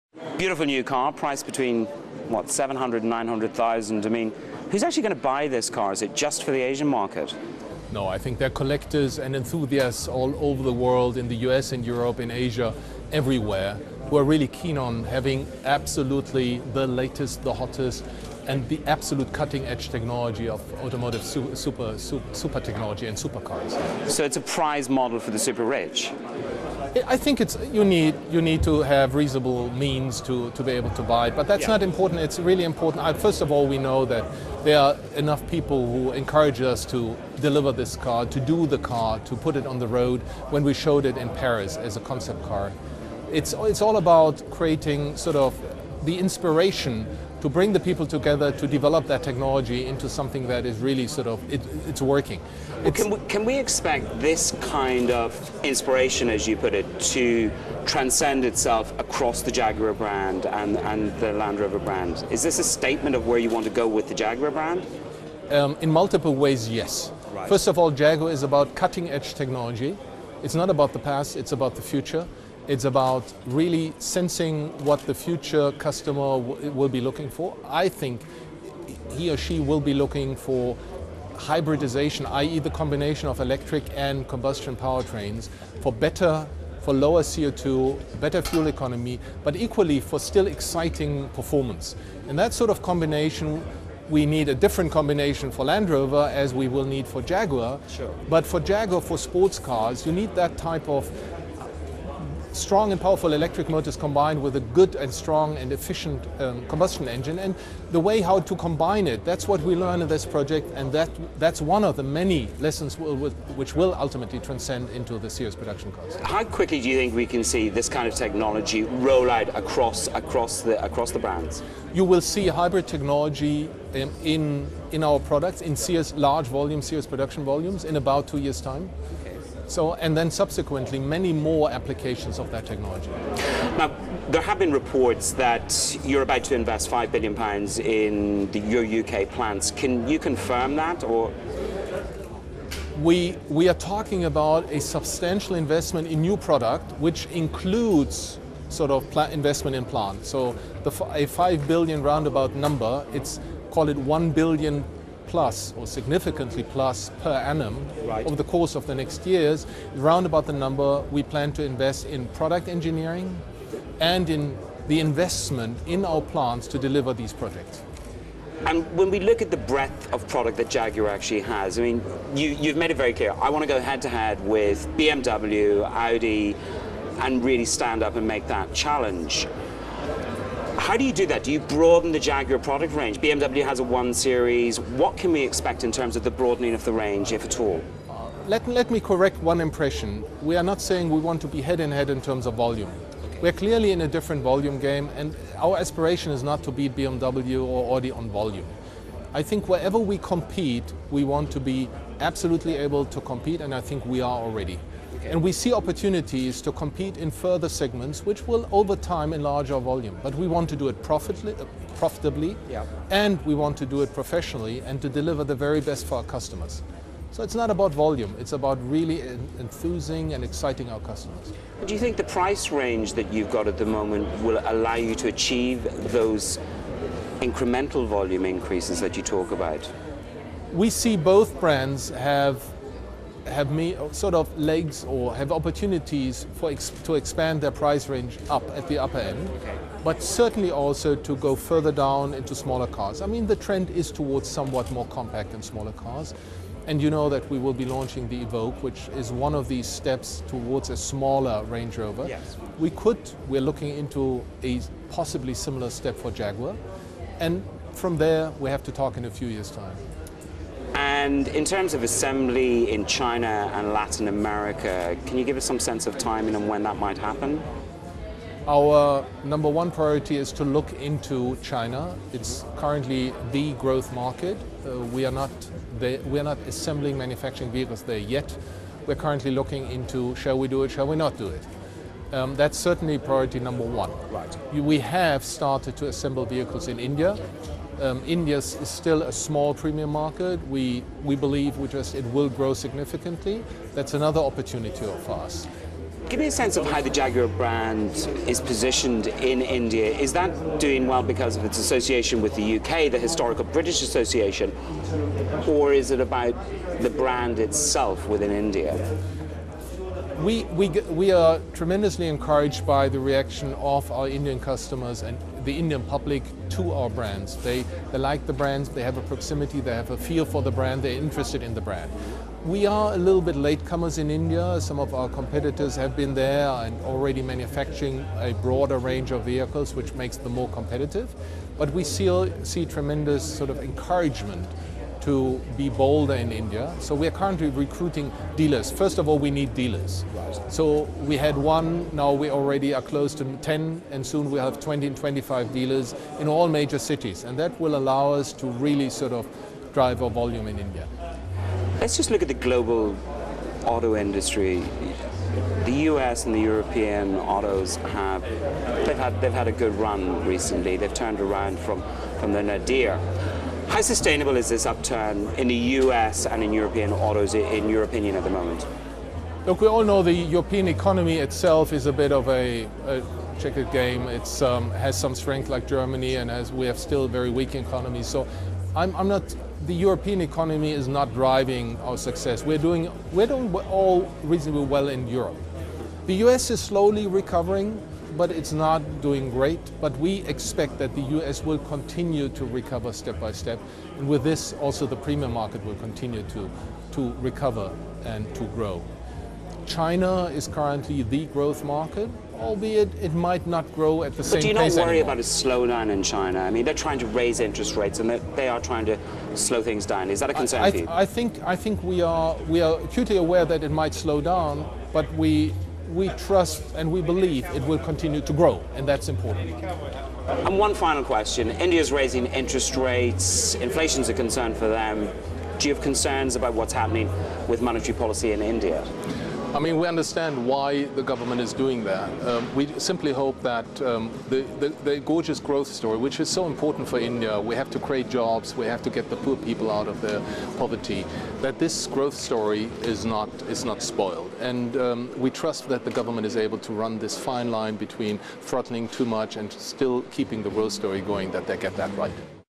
Forster Says China Is No.1 Growth Market for Jaguar
May 6 (Bloomberg) --- Tata Motors Ltd. Chief Executive Officer Carl-Peter Forster talks about Jaguar Land Rover's new hybrid supercar.
     The luxury-car unit of India's Tata Motors said it will begin production of the C-X75, an electric supercar priced from 700,000 pounds ($1.2 million) and able to reach 60 miles per hour in three seconds. He speaks with Bloomberg's Manus Cranny at a press conference in London.